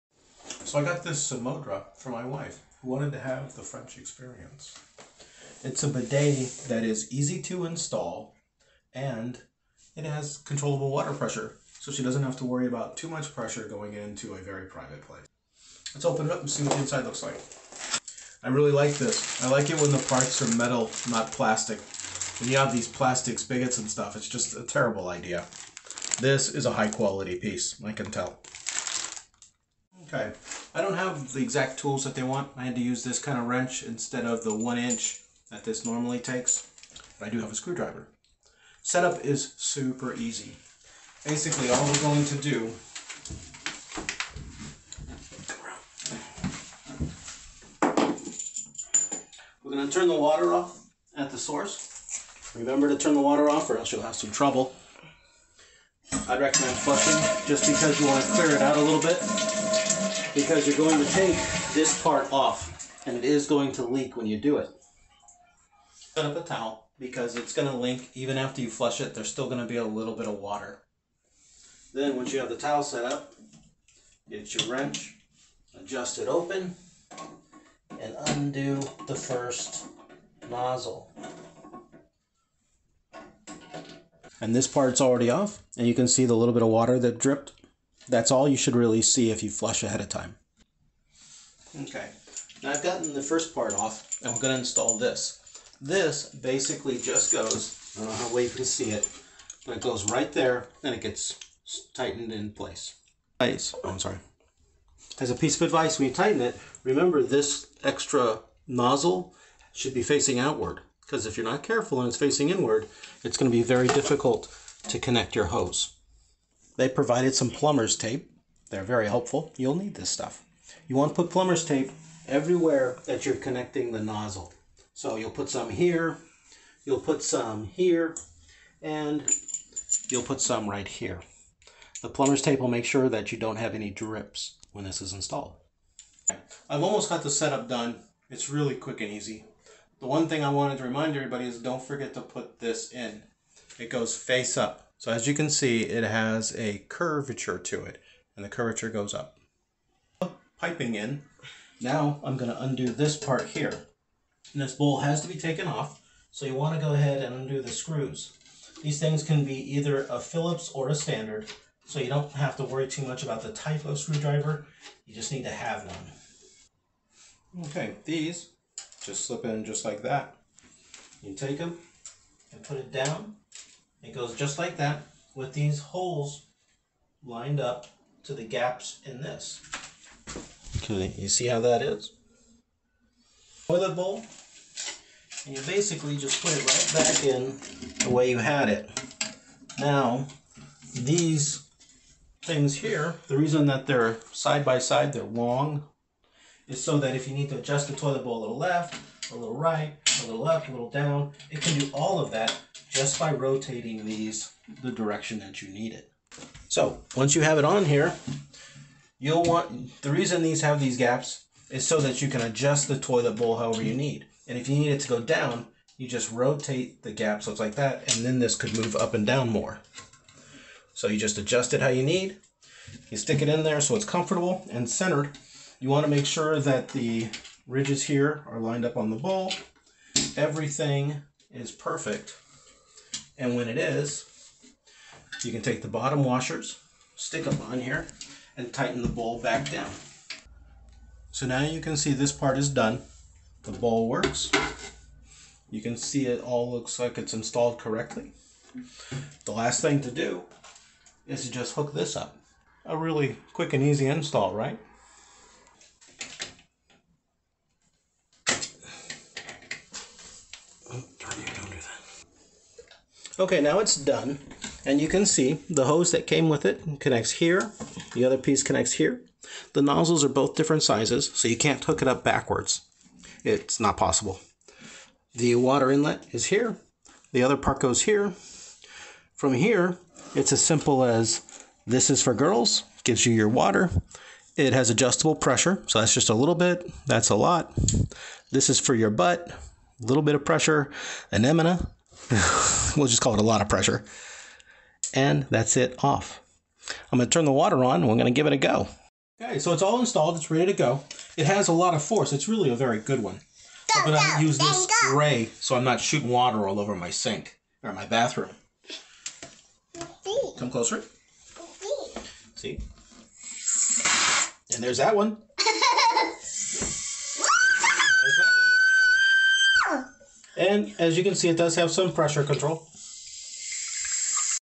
Samodra Minimalist Ultra Slim Bidet Real Review
Samodra - Everyone needs a bidet.
We got a video of a Samodra fan who received the Minimalist bidet from his wife.It can be seen that he and his daughter like this gift very much.

00:17 Unboxing the samodra dual nozzle bidet
00:30 The wrench and screwdriver
00:45 How to install bidet spray
02:28 The washer should be face up
04:40 Samodra bidet attachment feautre
04:58 Go the bidet (Adorable)

Samdora customer reviews 2